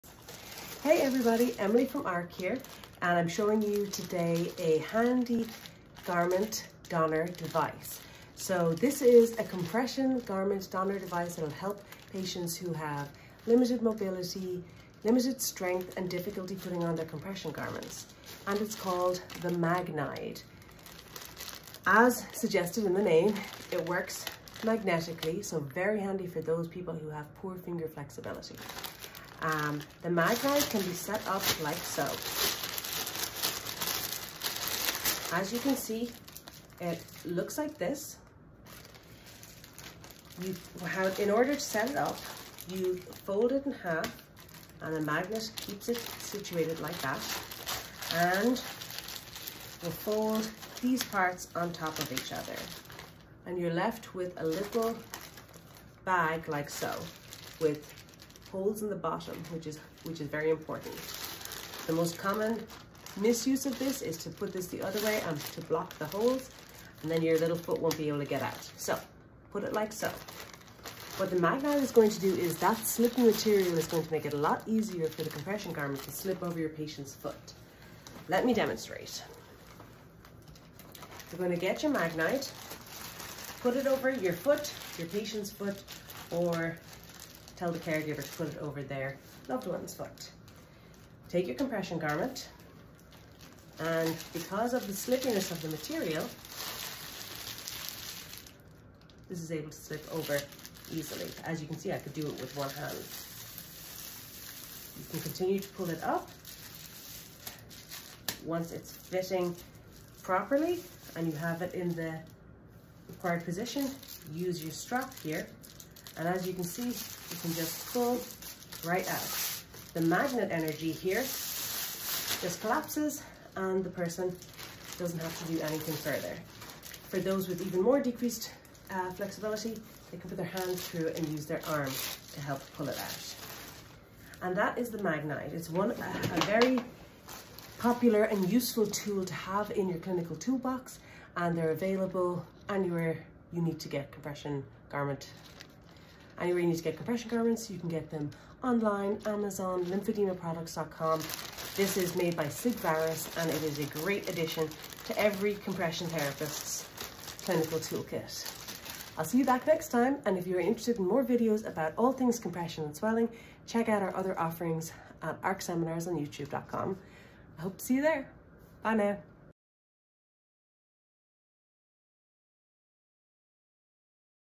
How to Easily Don Compression Garments: Magnide demo
Emily from ARC Seminars here again!  As a CLT, I know that donning compression garments can be a sweat-inducing couple of minutes for you and your patients.  The Magnide is an ~awesome~ little magnetic adaptive aid that makes the entire process so much easier!  Watch my demo here & upgrade those edema management skills